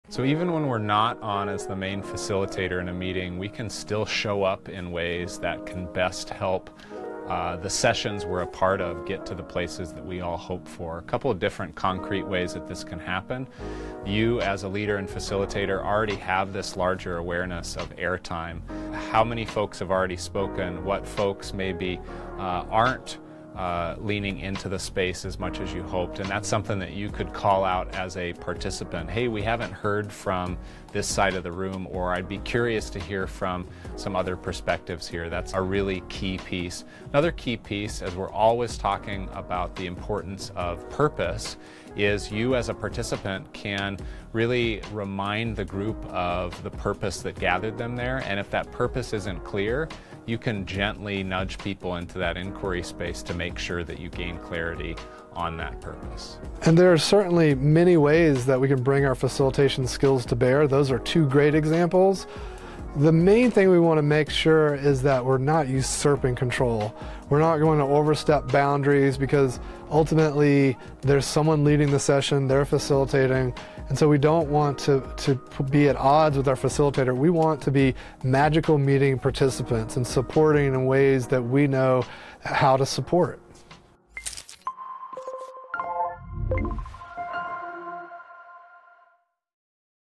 Facilitating As A Participant
Every voice in a meeting or workshop holds the power to guide and inspire. Facilitating isn't reserved for those at the helm; as participants, we can steer discussions, foster inclusivity, and contribute to meaningful outcomes. Be active, be engaged, be a facilitative force.